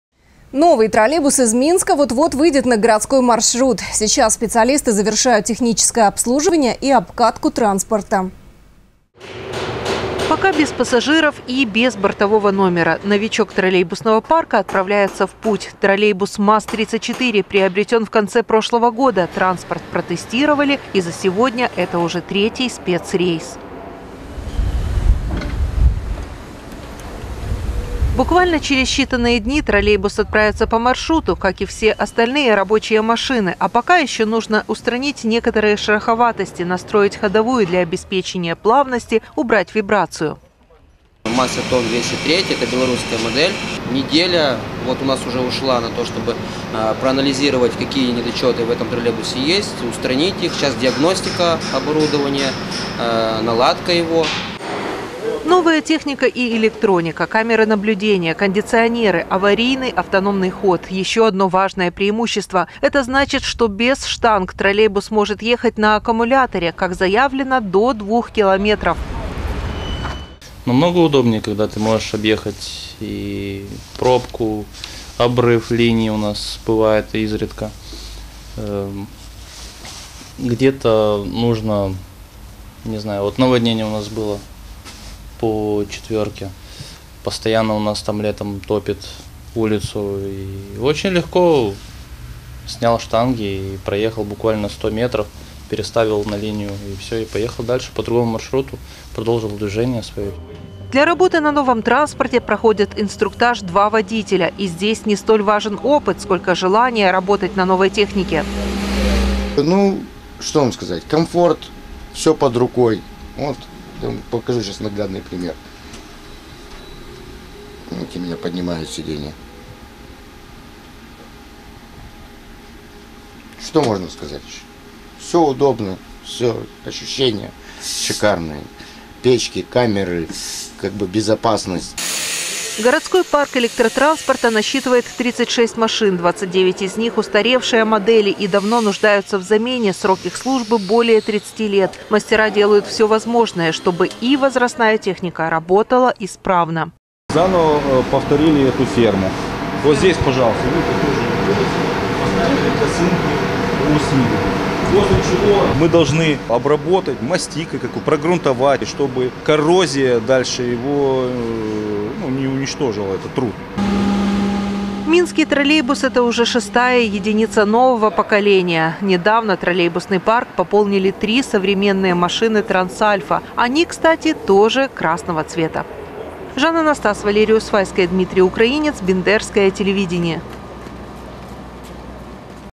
По свежему снегу – в добрый путь
Новый троллейбус из Минска вот-вот выйдет на городской маршрут. Сейчас специалисты завершают техническое обслуживание и обкатку транспорта.